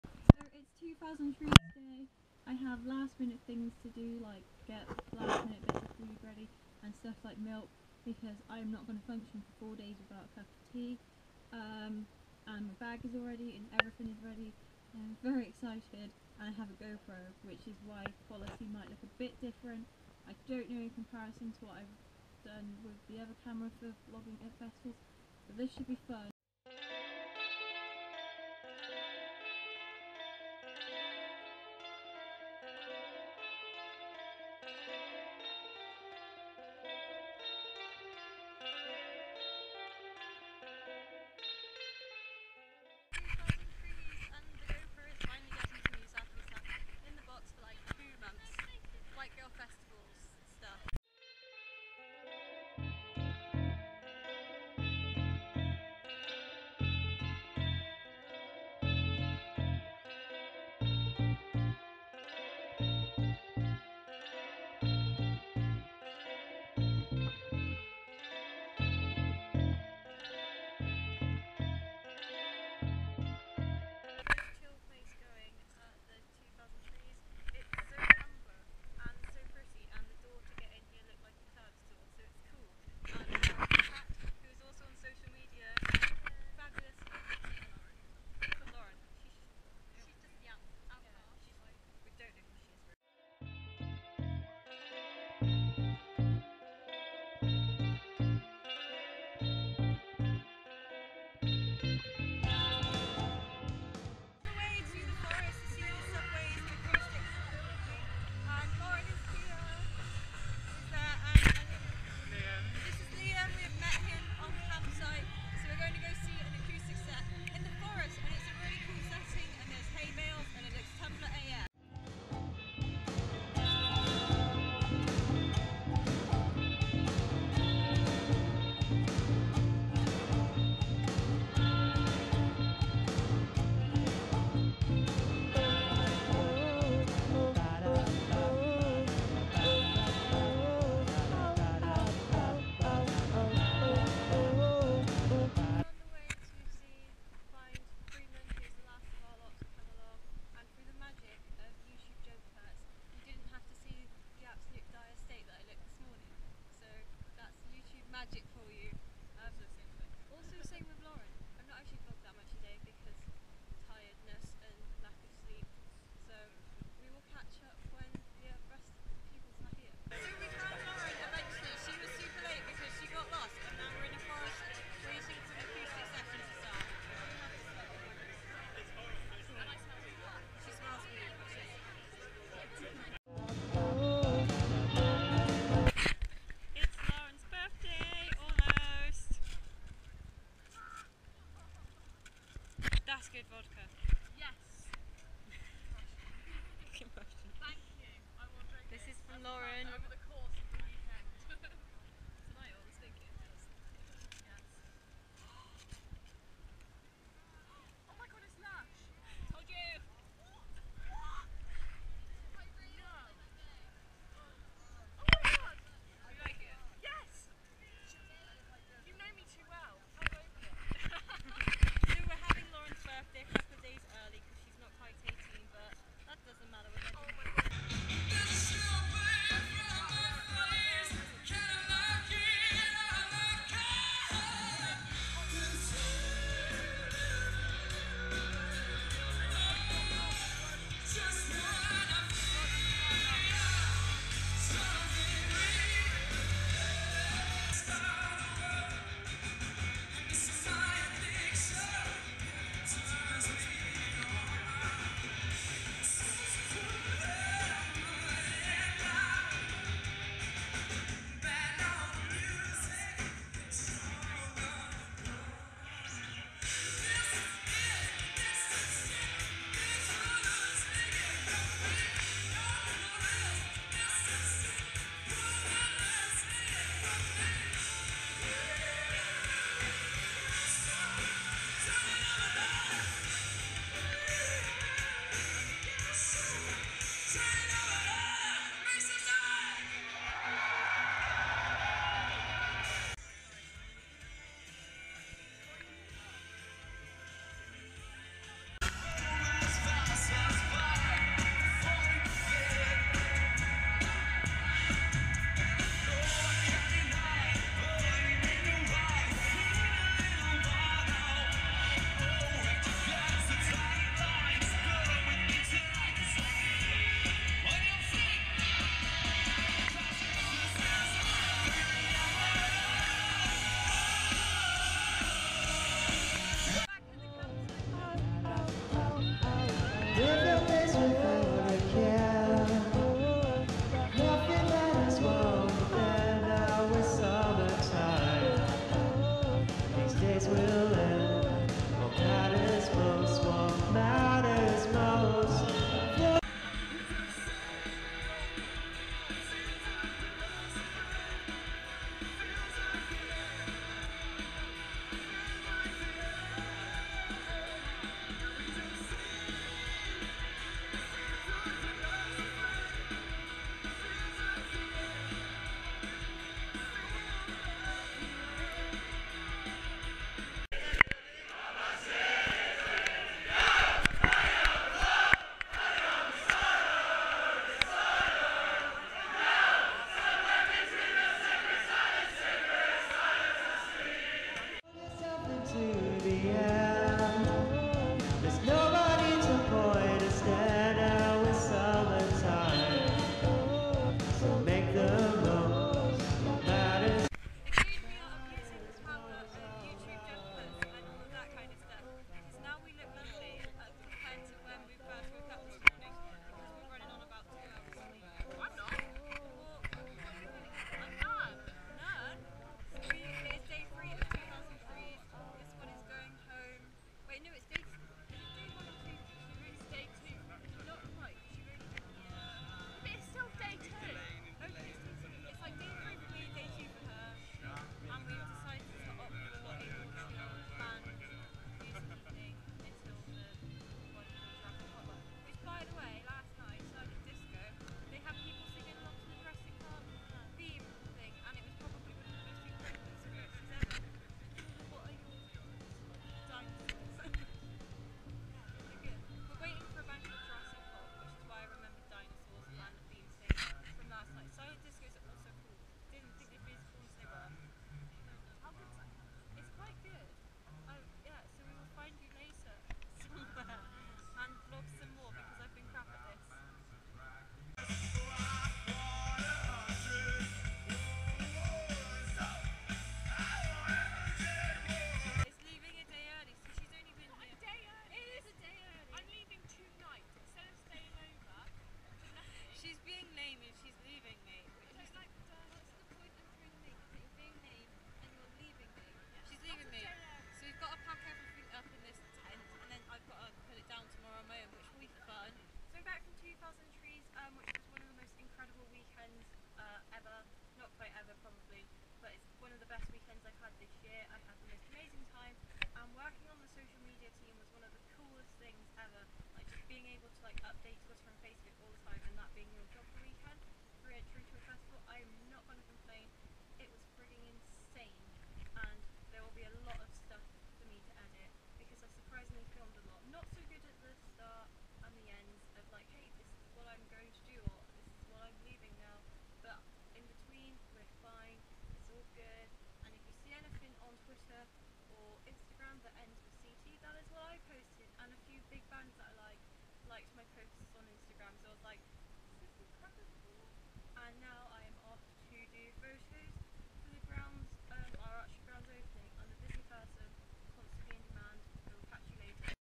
2000 Trees 2015
Quite possibly the worst editing skill to date. I spent a weekend working on the social media team at a music festival and it was super cool. Heres a video of it

Music by the wonderful James Missin, check him out @ these places